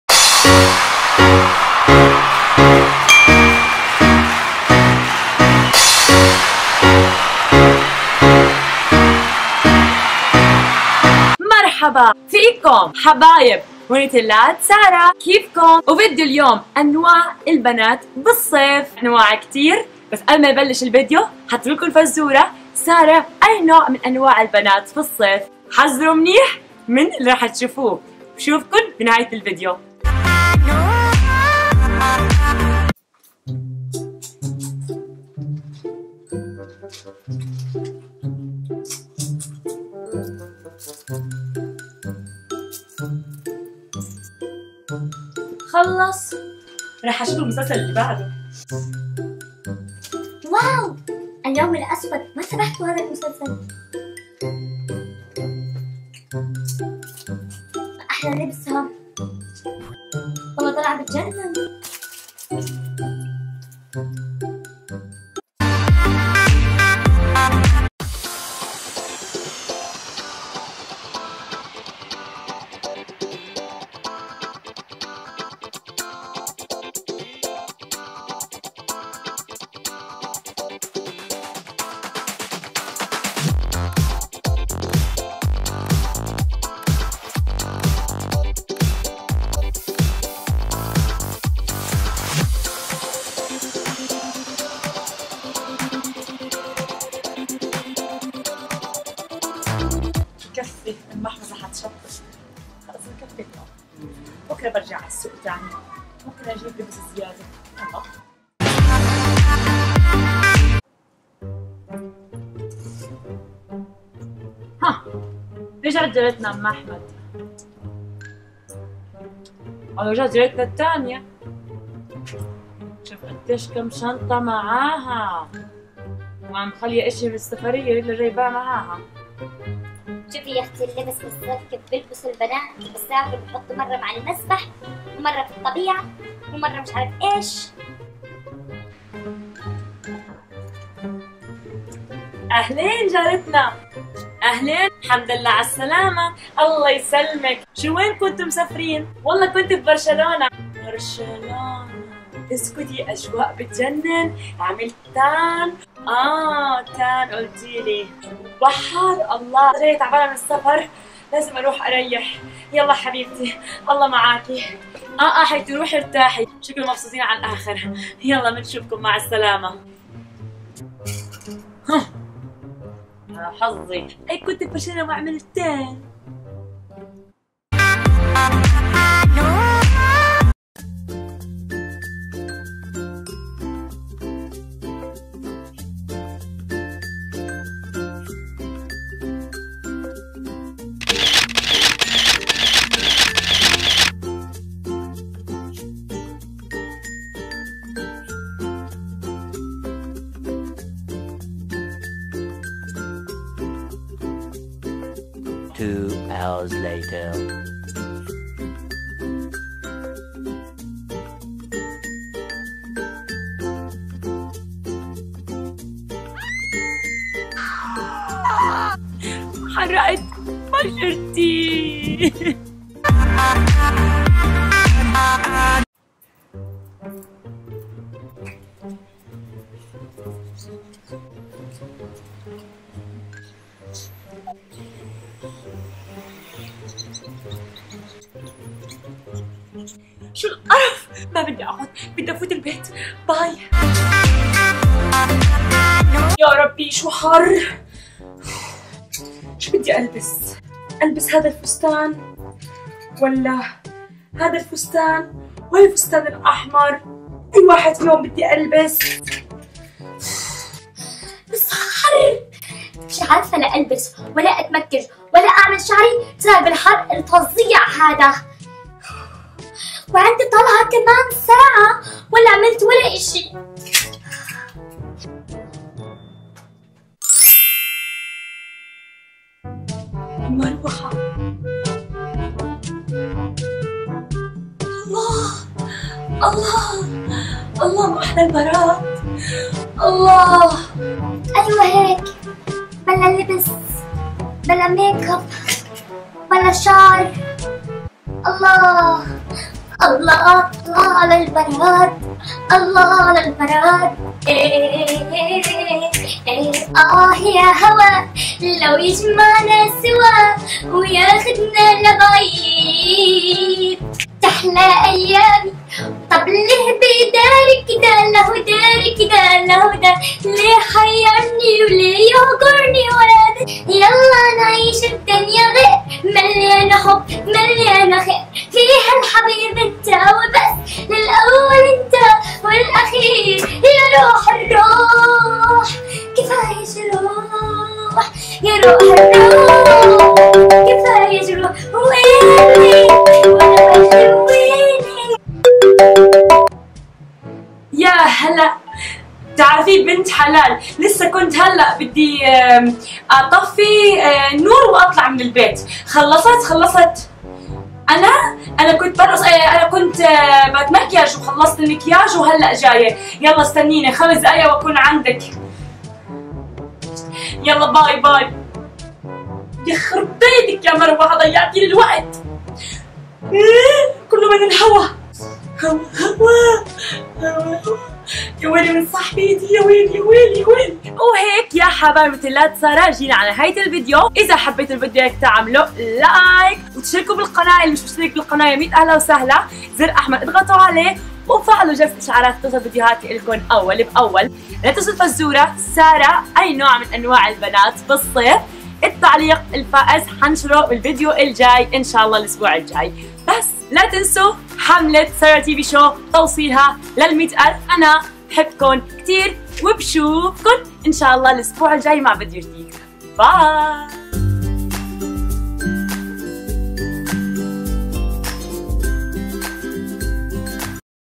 Types Girls In Summer ! ☀️ انواع البنات في الصيف
البنات الهم انواع كتير بالصيف حابيت اطرح بعض الانواع بطريقة كوميدية بناء على طلبكو ان شالله يعجبكو😄 لا تنسوووو الايك 👍🏽